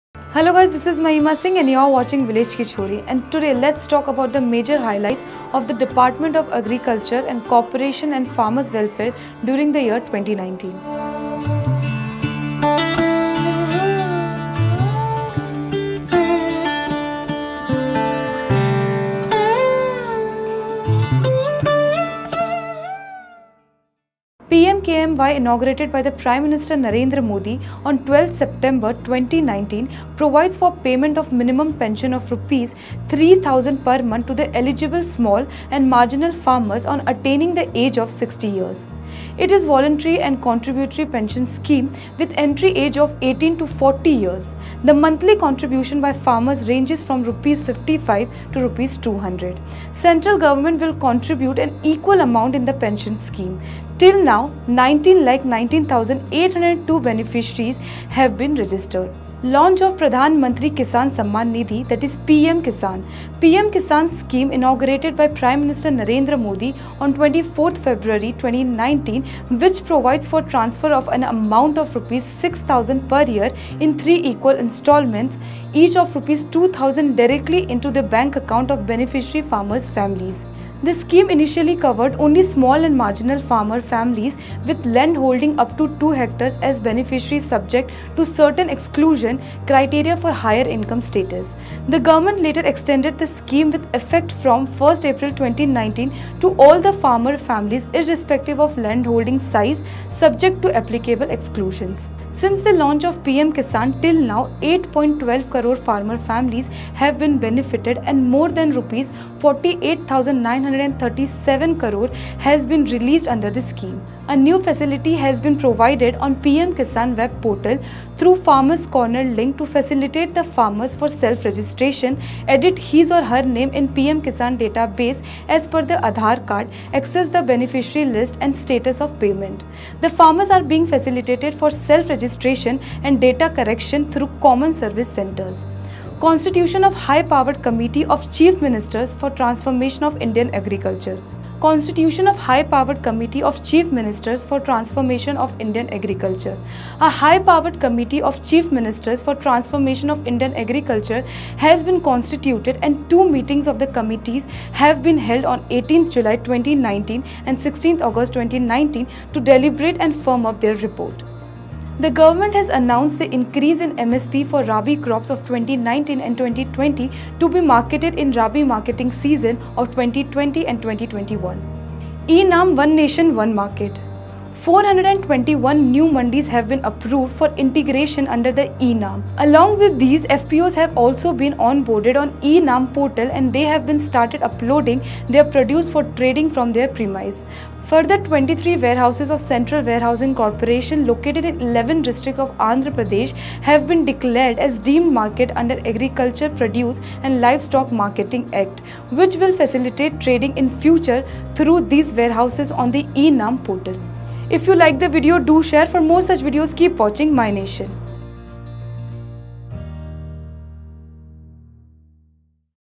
Major Highlights of Govt Schemes For Farmers During the Year 2019
PM-KMY inaugurated by the Prime Minister Narendra Modi on 12th September 2019 provides for payment of minimum pension of Rs.3000/- per month to the eligible small and marginal farmers on attaining the age of 60 years.